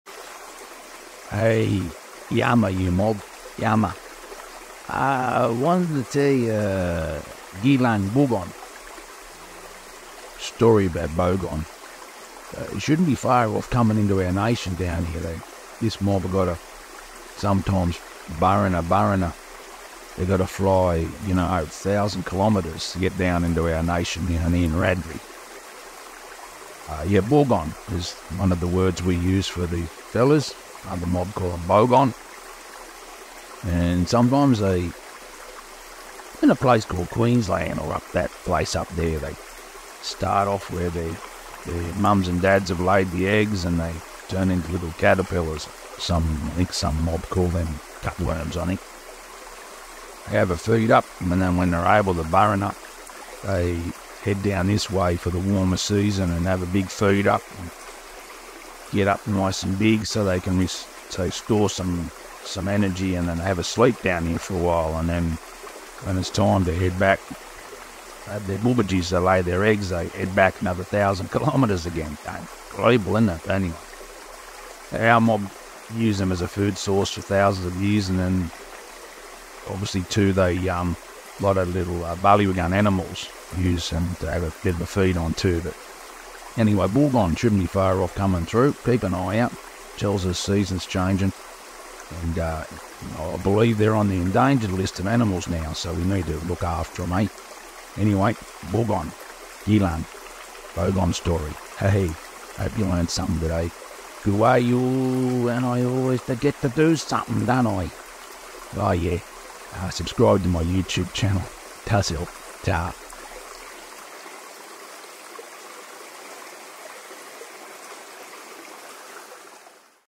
Bogong Moth Story, Australia.
Buugang ( Bogong Moth) Wiradjuri word,

Adult buugang moths have an overall dark brown colouration, with a dark stripe seperated by two light-coloured spots on the wings, distinguishing it from other moths.

Buugang moths lay up to 2000 eggs in the soil or on plants near the soil after returning from their summer visiting sites.

The Buugang moth has just been added as endangered to an international red list of threatened species.

Their populations have been declining since the 1980s, but in 2017 and 2019 there was a crash in numbers because of drought in regions where moth larvae grow: leaving them without the nutrients from plants.

Hope my short video has helped you in some way to better understand Buugang ?

Please "Subsribe" to my You Tube Channel.

It helps me to share more giilang (story)

Video: Mark Saddler
Voice Over: Mark Saddler
Animated Animals: Mark Saddler